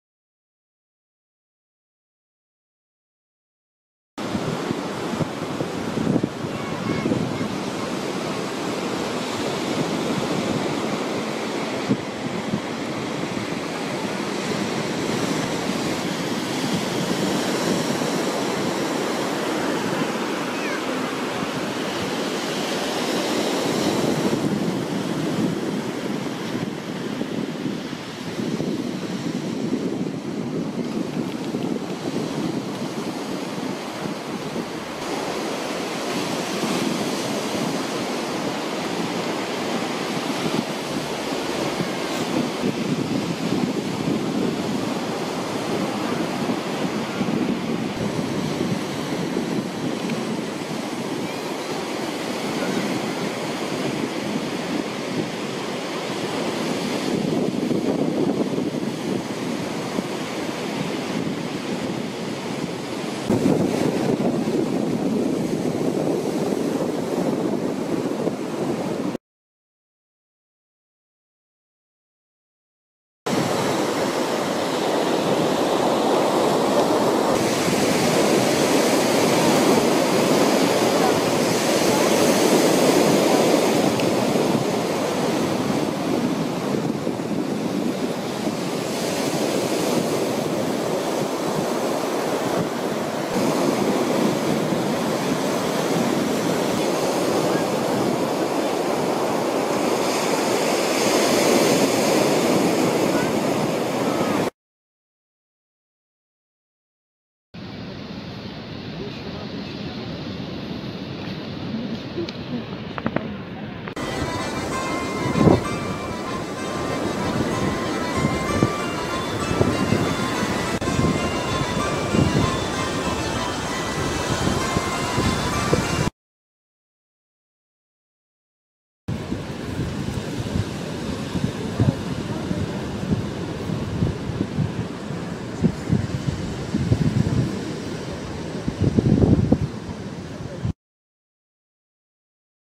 Одесса, Лузановка. 07.10.2018г.
Одесса, пляж Лузановка. 7 октября 2018г.

Odessa, Luzanovka beach. October 7, 2018.